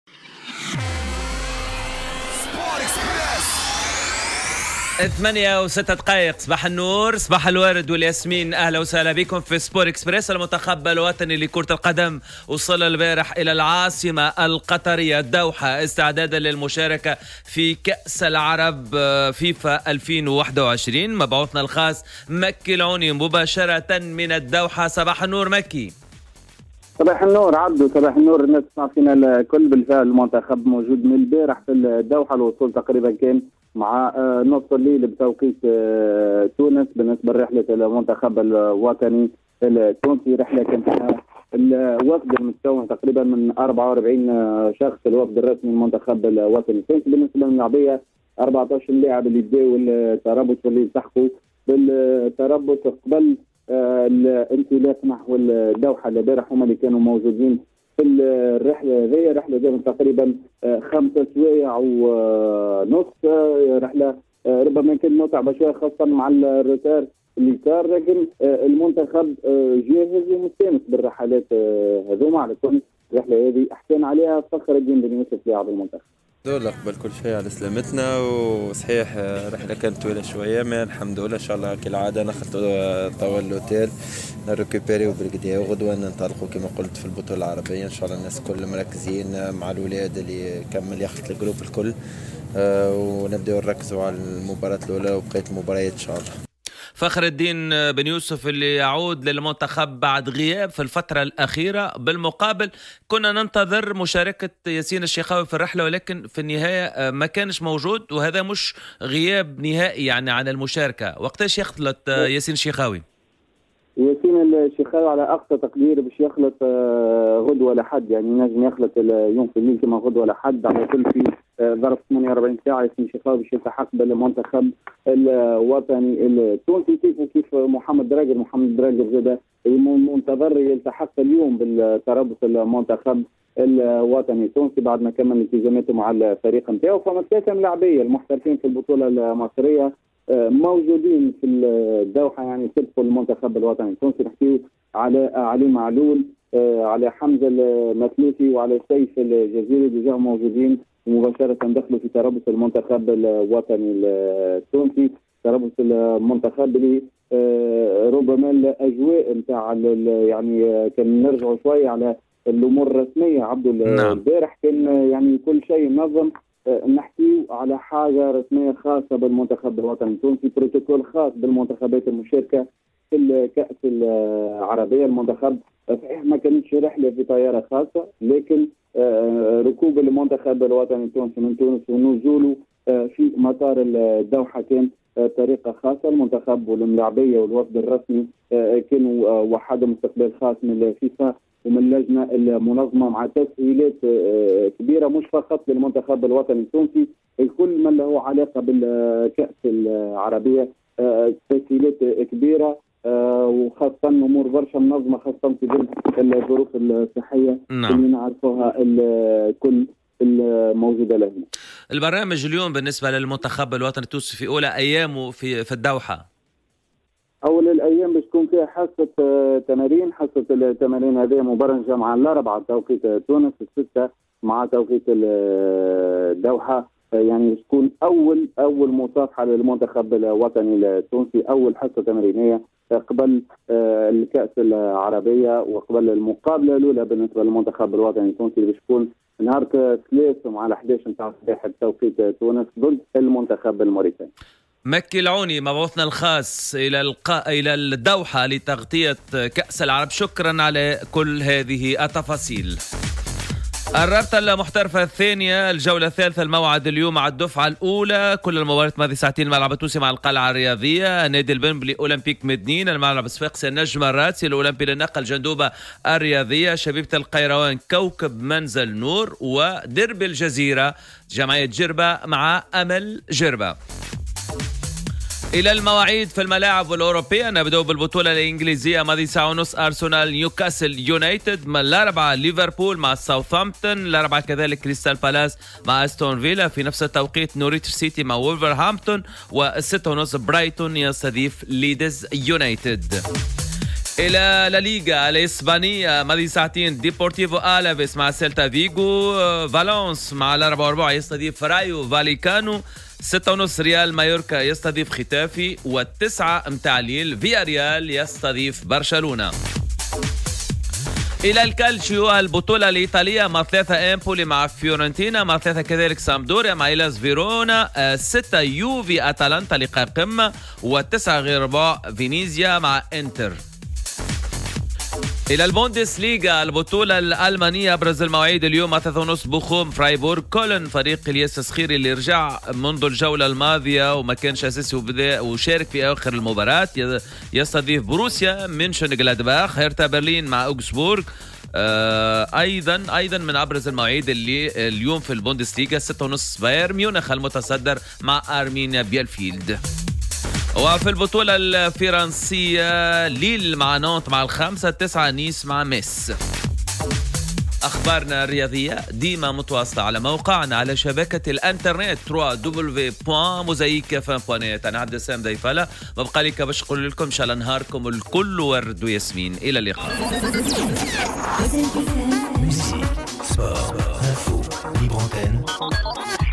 Sport Express : تفاصيل رحلة المنتخب الى قطر واش قال فخر الدين بن يسوف
Sport Express : Les dernières nouvelles de l'EN à Doha